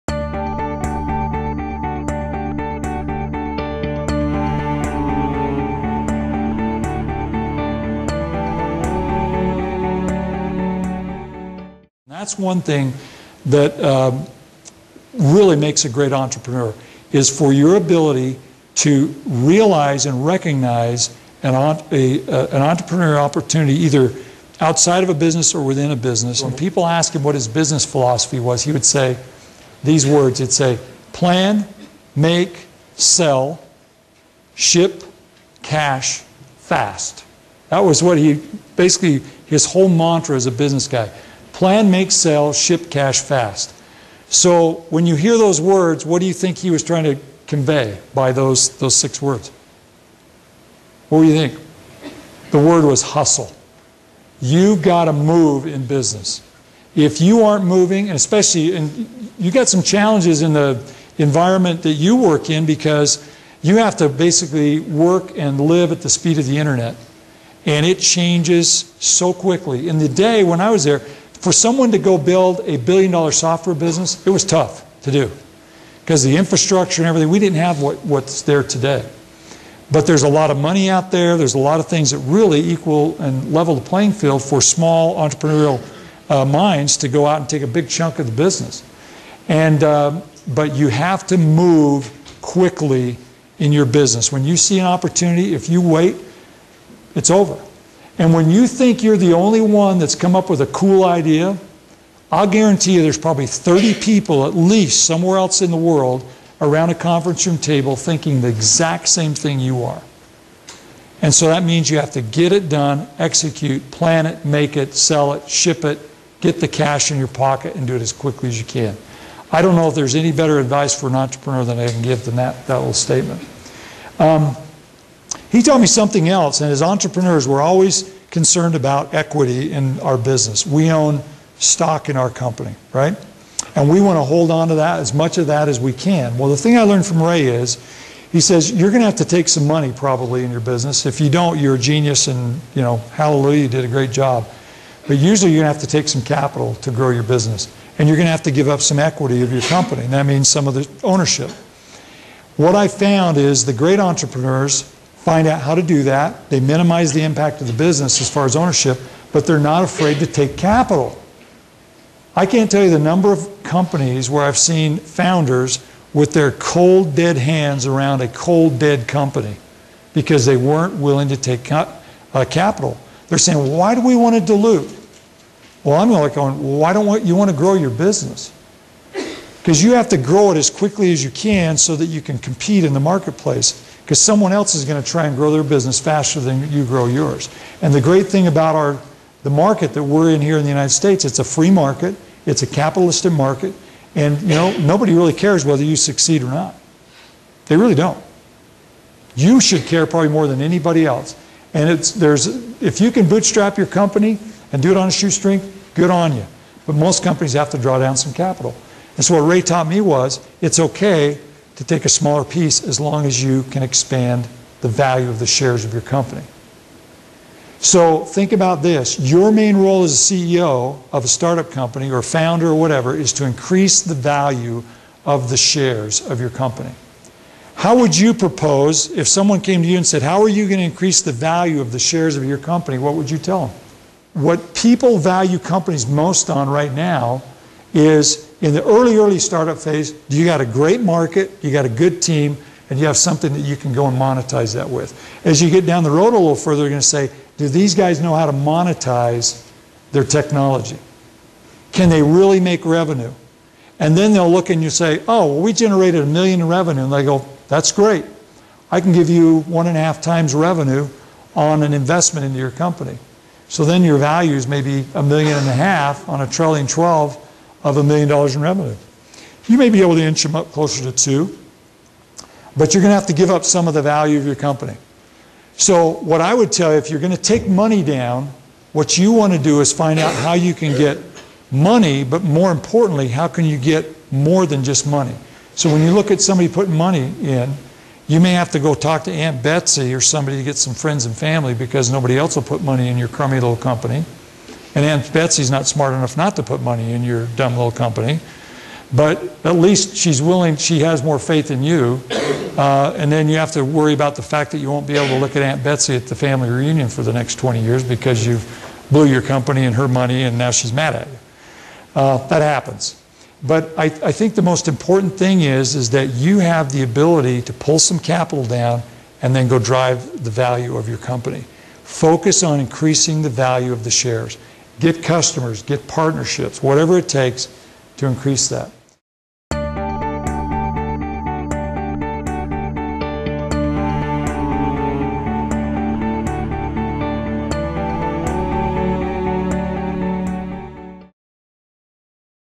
Jan Newman - Move Quickly in Your Business - March 7, 2011
Jan Newman, Founder of Altiris, gives advice to entrepreneurs and new business owners. His general theme is to move quickly in innovation.